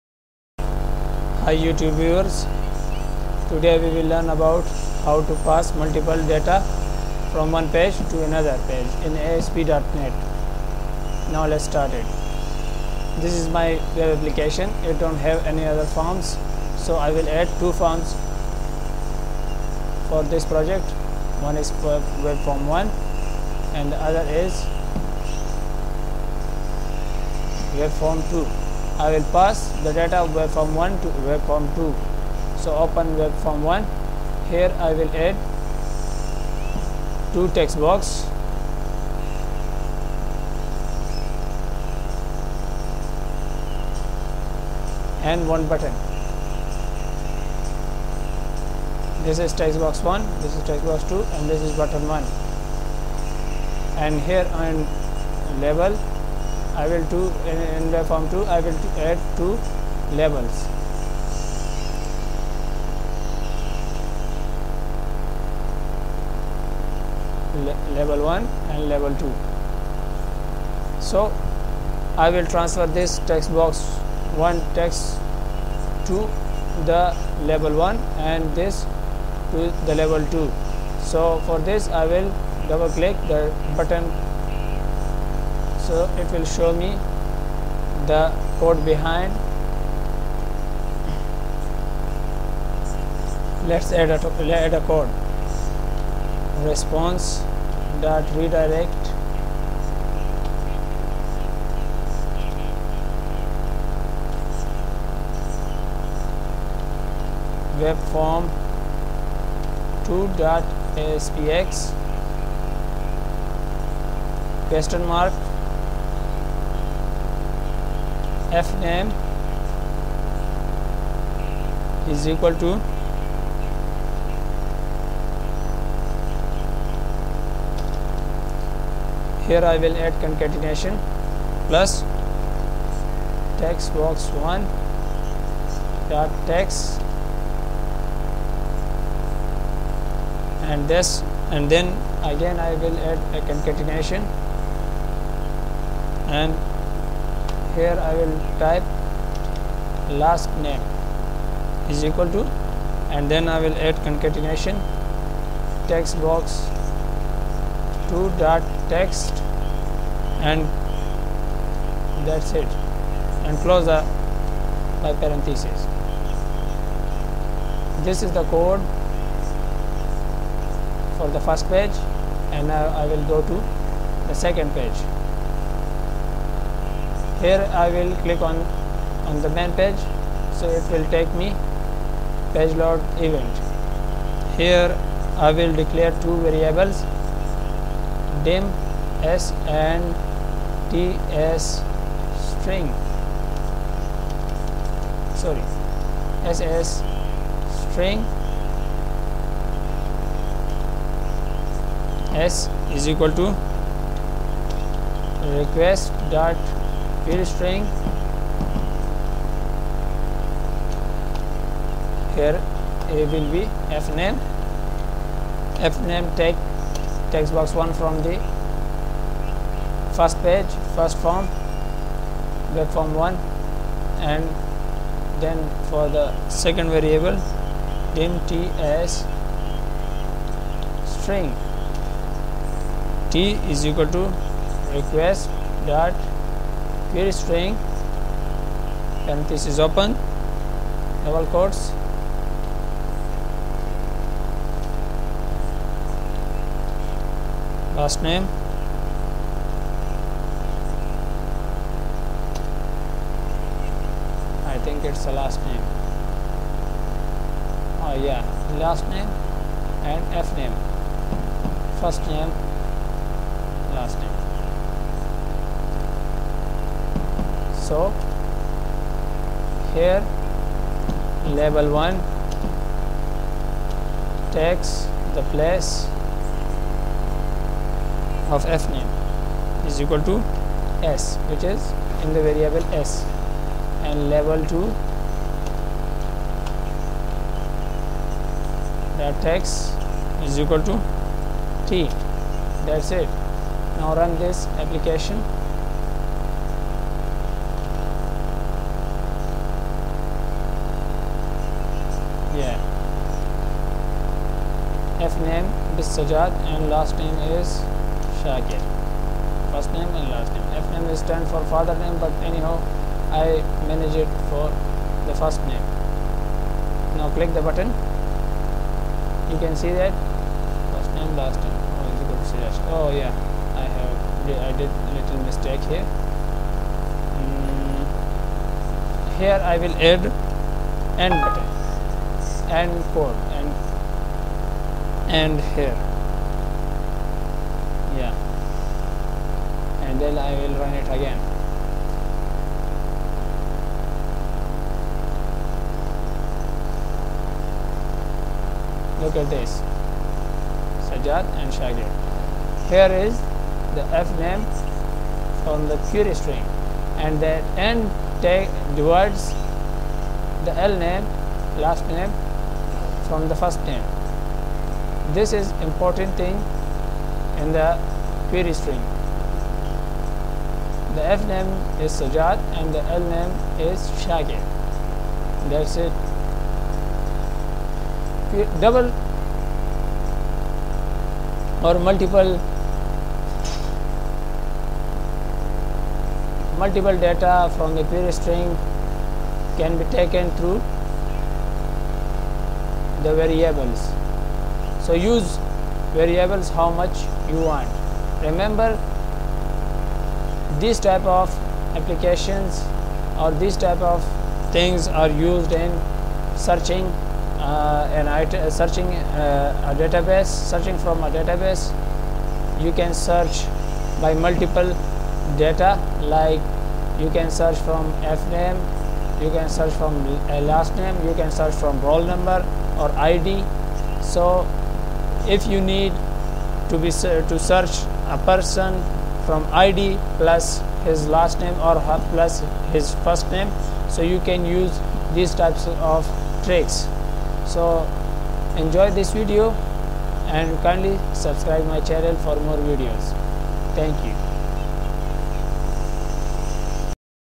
Asp.net Request.QueryString tutorial how to pass multiple data from one page to another "Vb.net""
A Very Simple Tutorial in which you can pass multiple data from one page to another using request.querystring .
please ignore my mistakes coz i am not so professional .. just take your advantage from the trick ..
also msg me if you have any good idea for me as well.